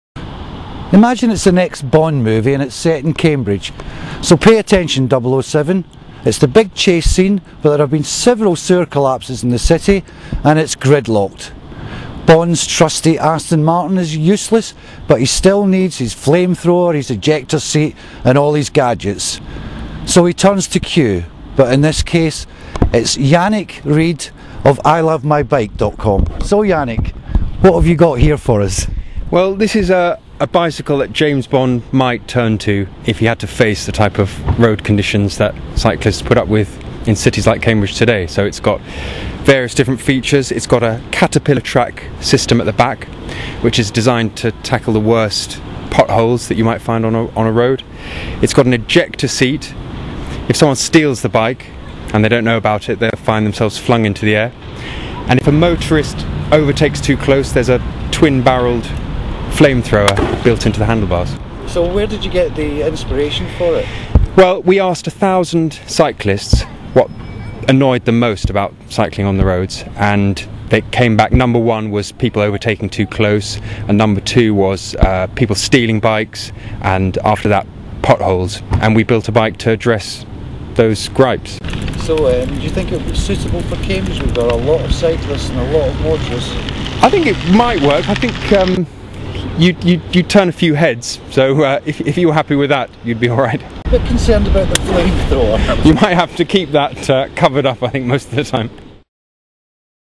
The James Bond bike
Ever wanted a bike fitted with an ejector seat or a flamethrower? A bike fit for James Bond is put through its paces on Parker's Piece in Cambridge - Follow this link for story